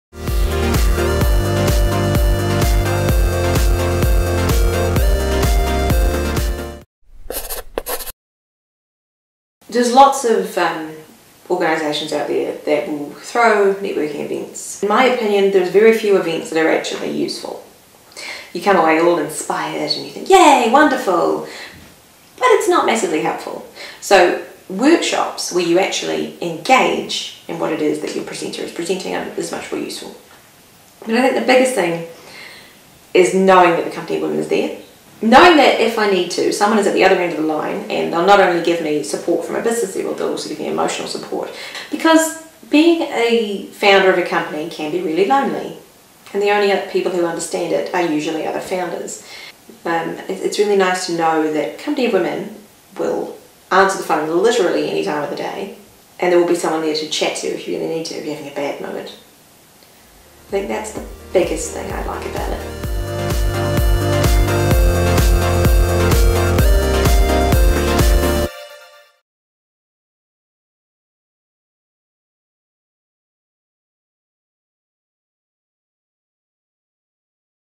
What I enjoy about my Co.OfWomen membership ft. Brianne West
Brianne West, the founder of Ethique, shares what she enjoys about being part of the Co.OfWomen community.